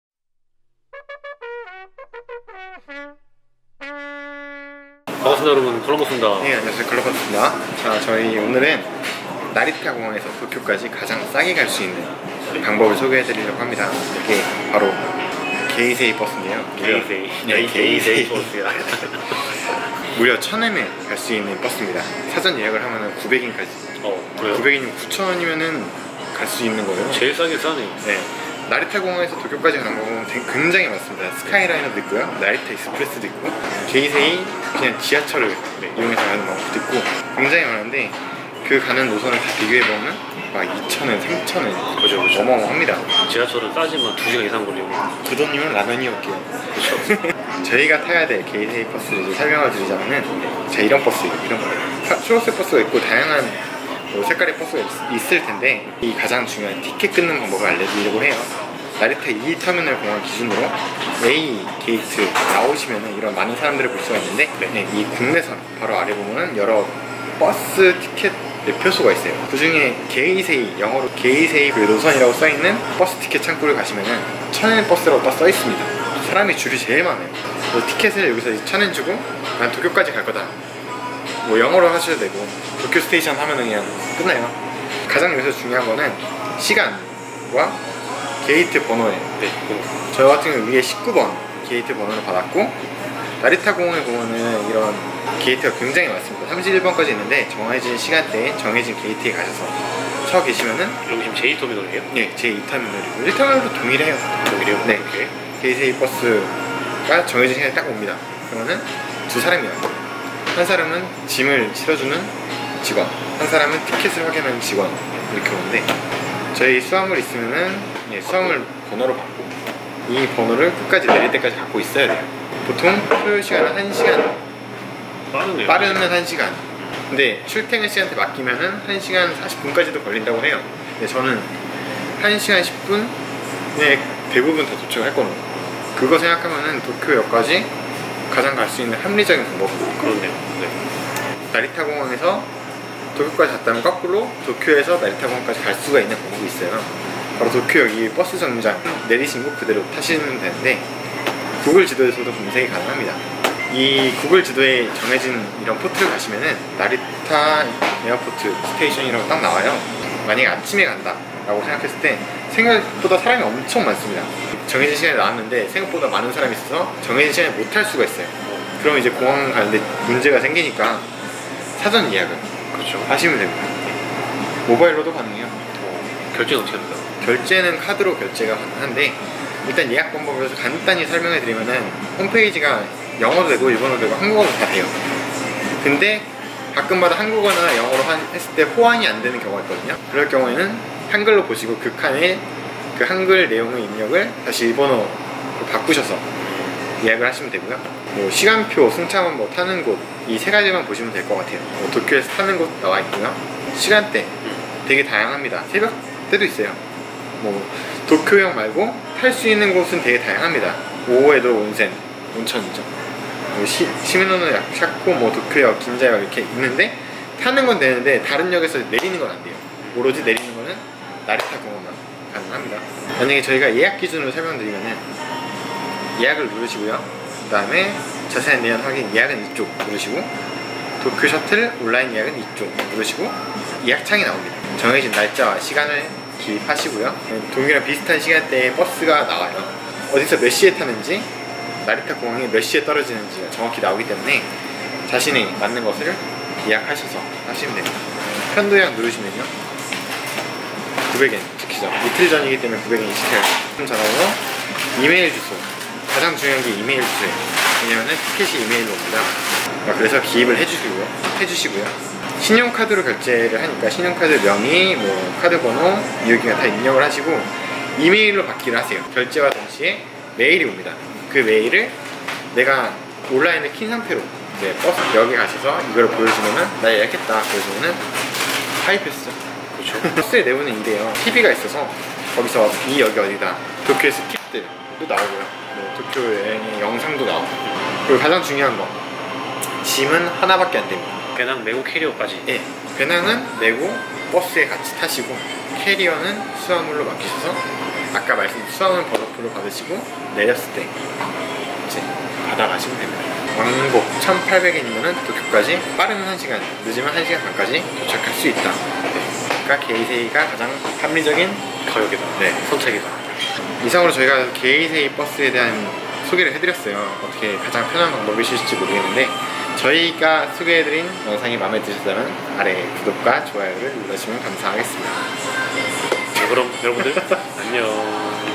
[컬럼버스TV] 나리타 공항에서 도쿄 시내까지 가장 싸고 빠르게 가는 방법 | 일본여행 | 꿀팁
안녕하세요 :)
컬럼버스 입니다~

오늘 영상은 나리타 국제 공항에서 도쿄 시내까지
가장 빠르고 싸게 가는 방법에 대해서 찍어 보았어요.

게이세이 버스에 대한 정보는 영상과
아래 URL을 통해 확인해 주세요 :)

*게이세이 버스 예약 웹사이트(한국어/영어 지원)

*게이세이 버스 도쿄역 탑승 위치(구글지도)

*나리타 국제 공항 웹사이트(크롭접속 한글 번역)

*컬럼버스TV 인스타그램